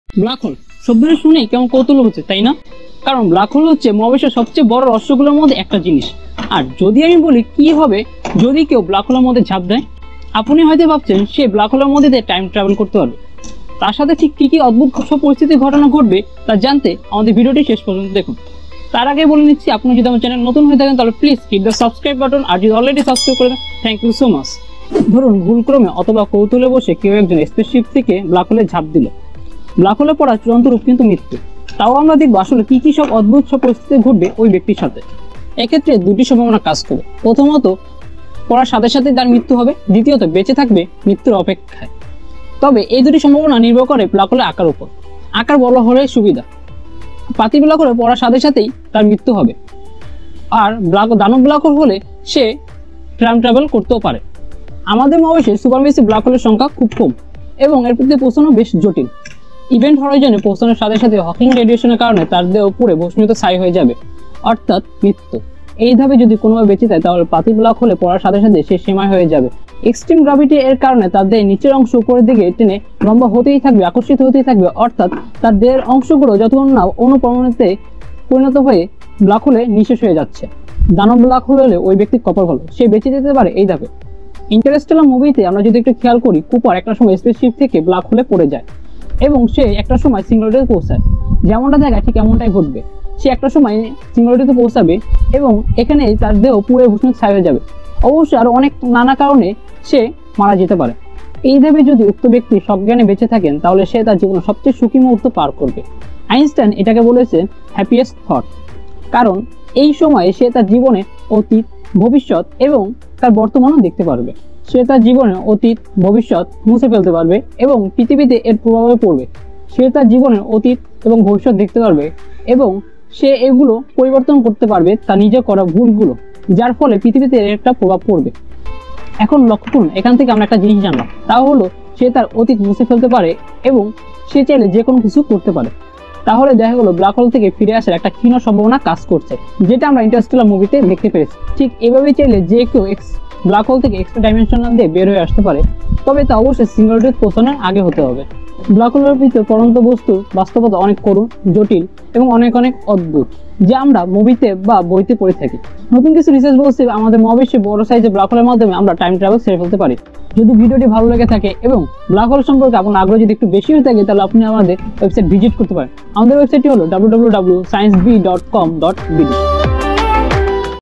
কী হবে যদি কেউ ব্ল্যাকহোলে ঝাপ দেয় ? | Blackhole | Science Bee
ভুলক্রমে অথবা কৌতুহলের বশে কেউ একজন Space Ship থেকে Black Hole এ ঝাপ দিলে
তার সাথে কি সব অদ্ভুত সব ঘটনা ঘটবে  তা নিয়ে এই ভিডিও তে বিস্তারিত বলা হয়েছে।

 ★★কী হবে যদি কেউ "ব্লাক হোল"এর ভিতর  ঝাপ দেয় ?

ব্ল্যাকহোলের ভেতর পড়ন্ত বস্তুর বাস্তবতা অনেক করুণ,জটিল এবং অনেক অনেক অদ্ভুত যা আমরা মুভিতে দেখি বা বইতে পড়ি তার চেয়েও।

Content Creator: Arif Hossain  Rajon
Video Editor: KHALIL ATIF KHAN